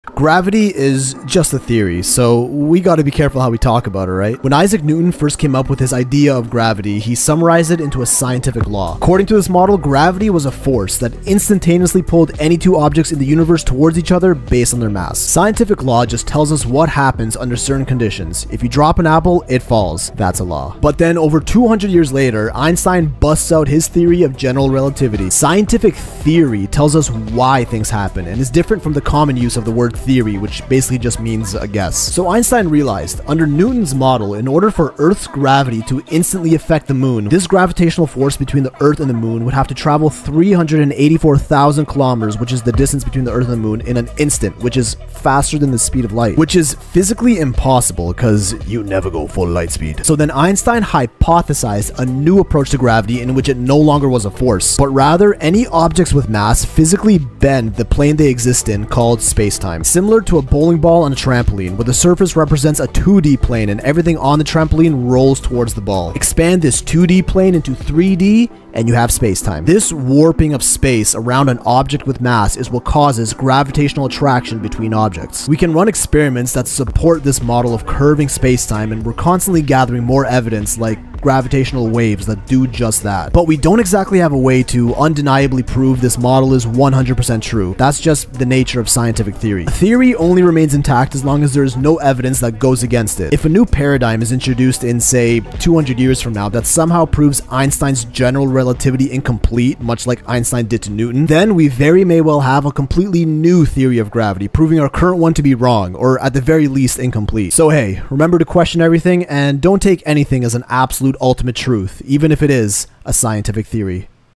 Why Gravity (or Any Theory) Can Never Be 100.00% True
"Only a sith deals in absolutes."

Theories are always evolving. Sometimes, they're proven incomplete, missing key components or outright fully disproven decades or even centuries later. It would be naïve to think that we know all the answers to everything with absolute 100% certainty simply because we're the most advanced humans in our historical timeline.

Always question everything. But yes, today gravity does exist.

FILIP ZIEBA (@filspixel) ON THE INTERNET